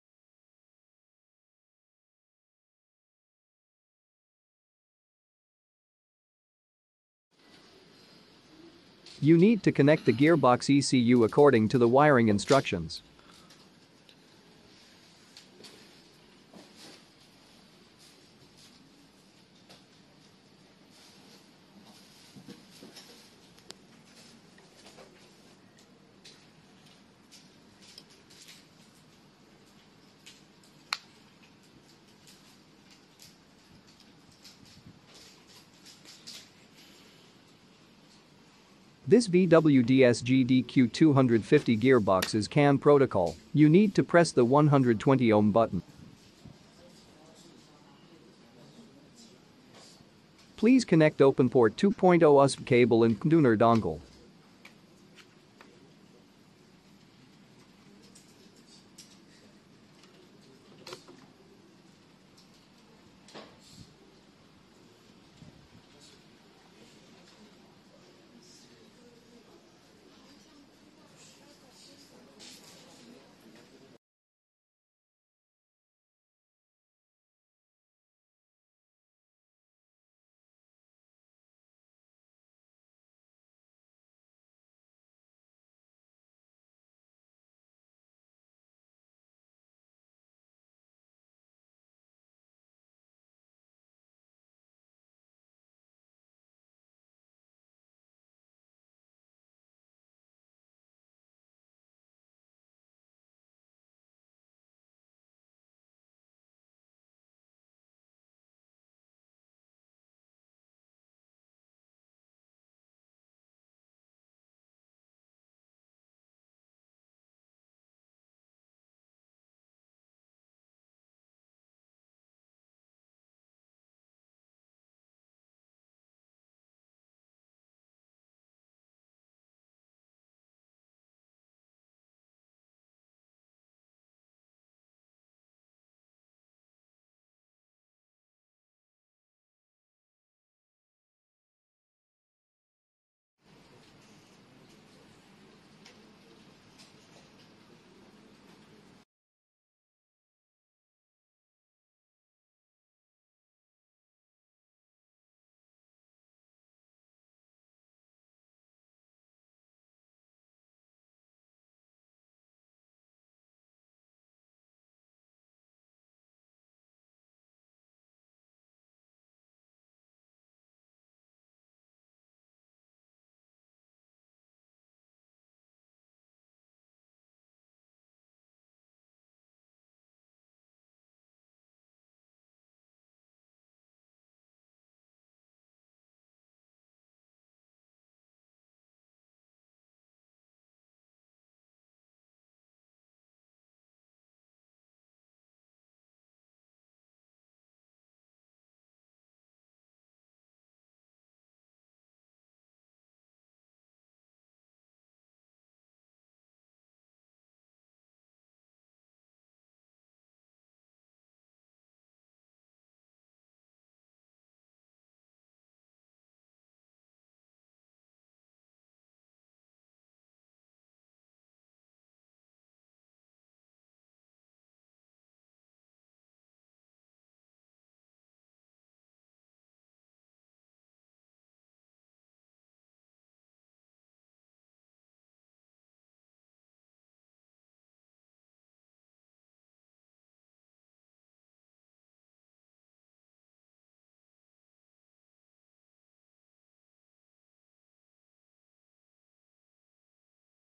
Openport 2 0 + Godiag GT107 read VW DQ250 dsg obdii365
how to use openport 2.0, pcm tuner usb dongle and godiag gt107 gearbox adapter to read VW DQ250 02E DSG gearbox ECU

pcmtuner dongle with 67 modules is compatible with original SM2 pro and openport.

GODIAG GT107 ECU IMMO Kit is a DSG gearbox connection adapter. DSG Gearbox maintenance engineers can clone, diagnose and repair DSG Gearbox ECU and can also read, write and adjust data for DSG Gearbox ECU via PCMTuner, and Kessv2, etc.

This adapter has automatic and manual selection for PCM software. For data reading and writing, you can choose “Auto”, and for diagnosis you can choose ”Manual”. For other software kessv2, DQ200, DQ250, DQ500 you can also choose “Manual”.

Skype: OBDII365.com